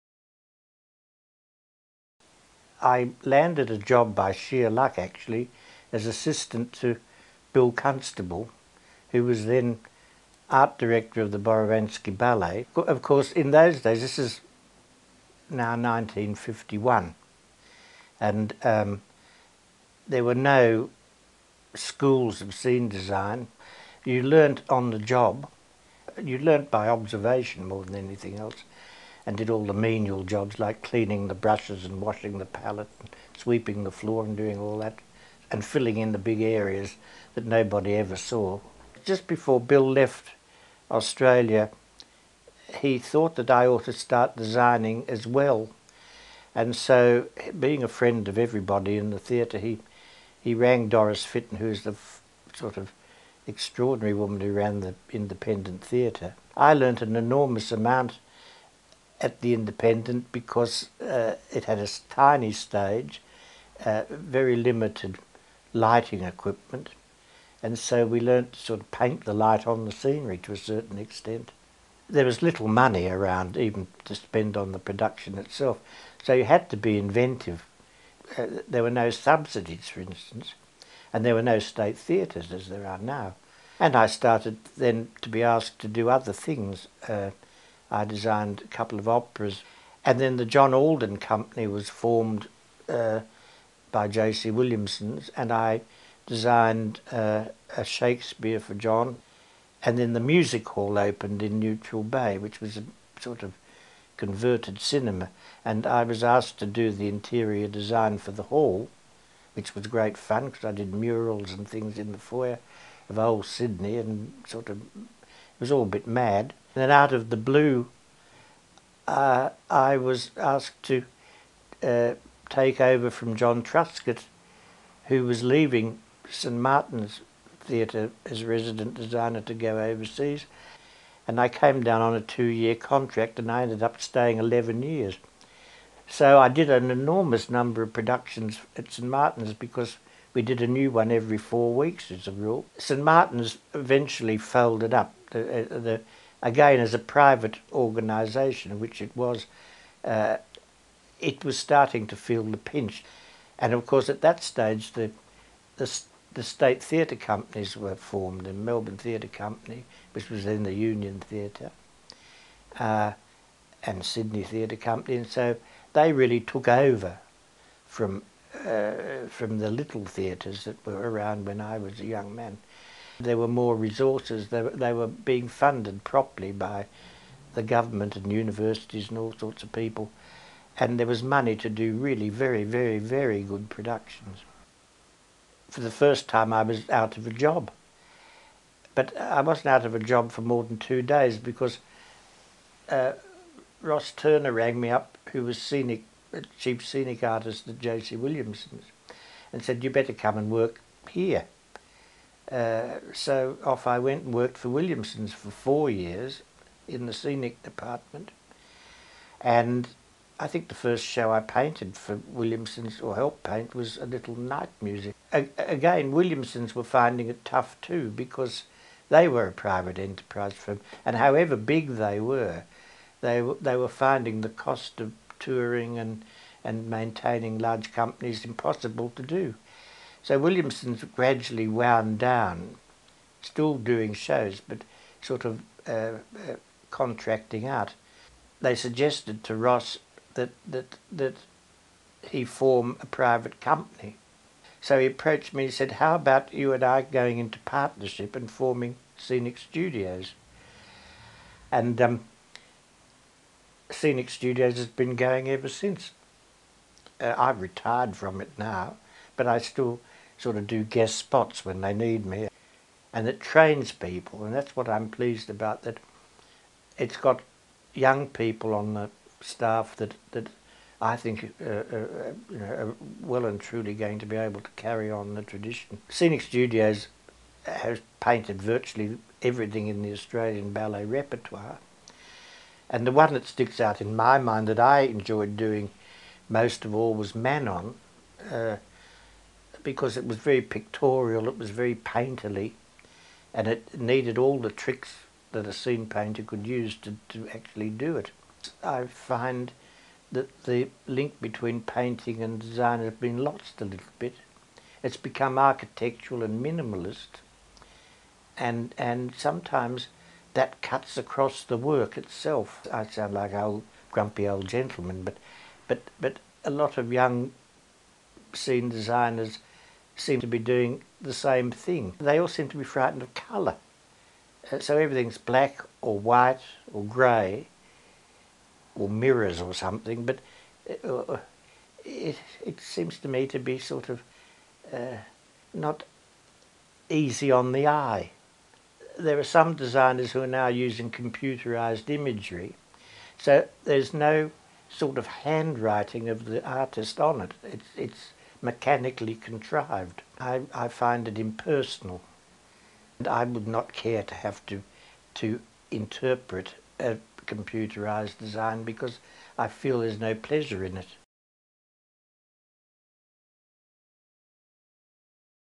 Paul Kathner discusses his career
Paul Kathner discusses his many years as a scenic artist in the Melbourne and Sydney theatre circuits and companies.

Cinematographer: Jackie Farkas
Editor: Sophie Boord

Copyright The Arts Centre, Melbourne